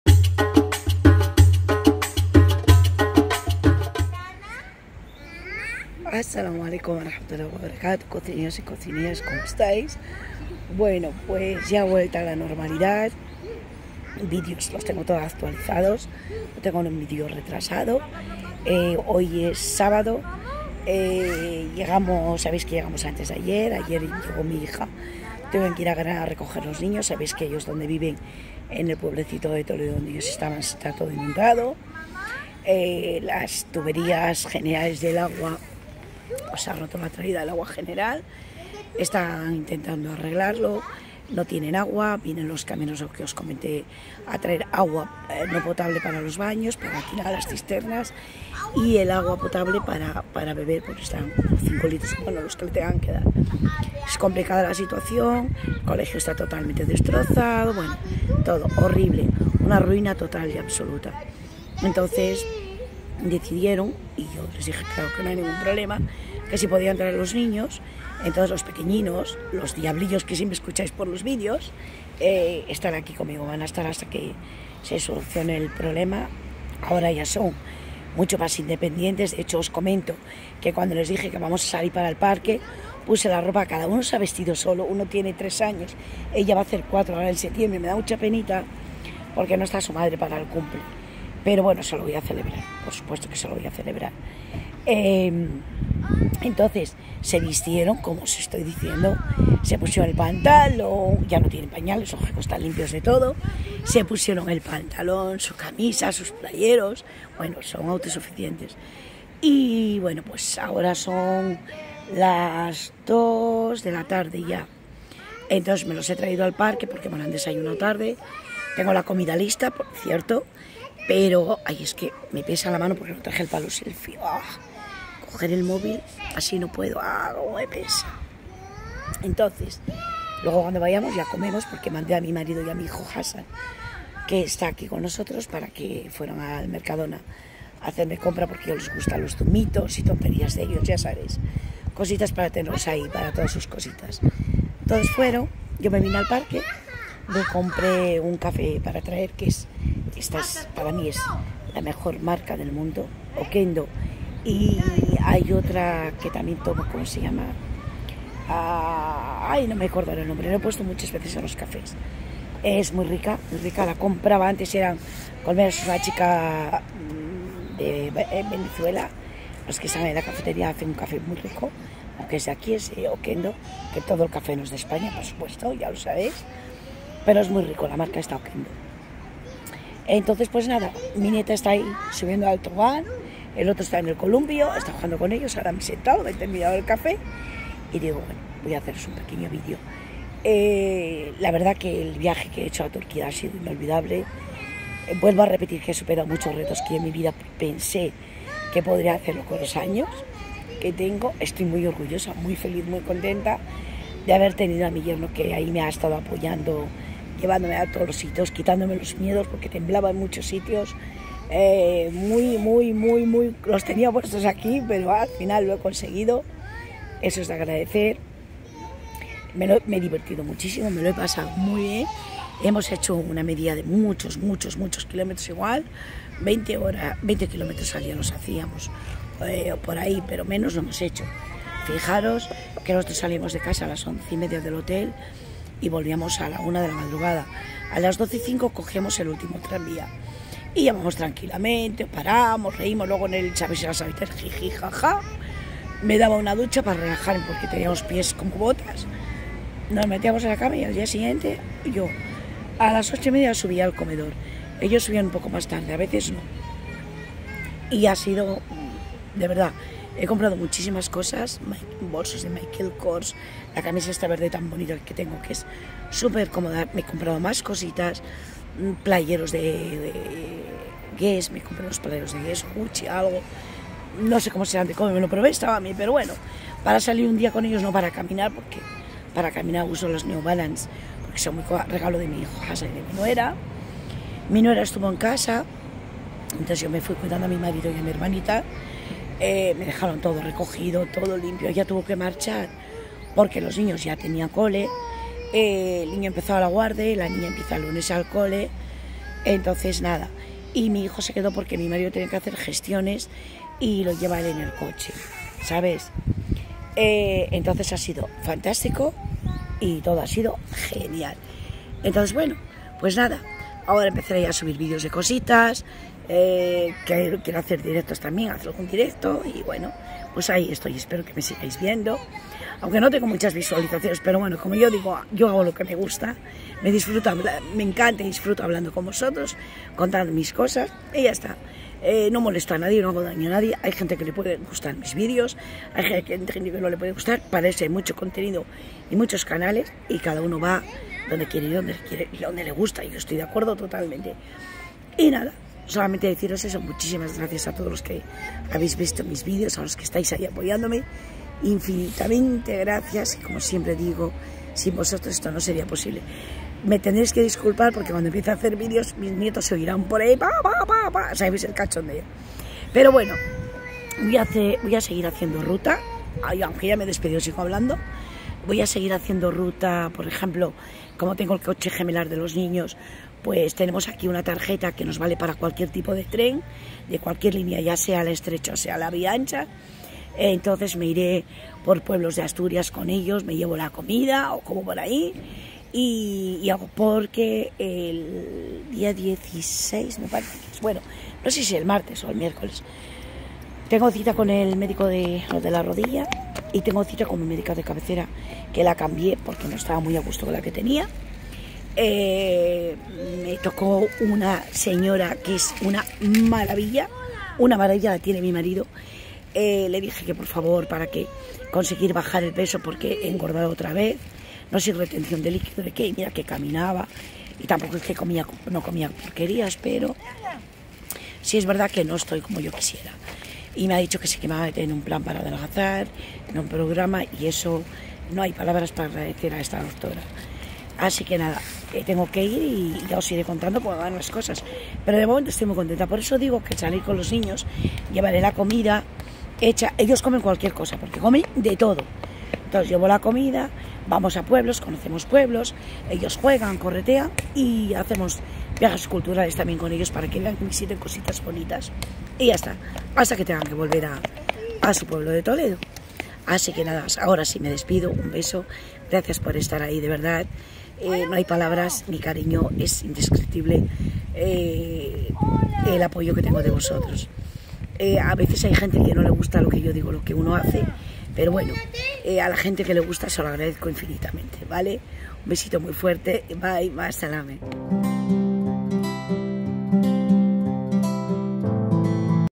VUELTA a la RUTINA + MIS NIETOS de TOLEDO VOLVIERON+FELICIDAD COMPLETA con la FAMILIA
Hola cocinill@s . Hoy vuelta a la rutina y  cuidando a mis nietos  abuela FOREVER. La situación en Toledo es muy complicada con las inundaciones. Felicidad al completo. Gracias por vuestro apoyo y a los que les gusta mi contenido un millón de gracias.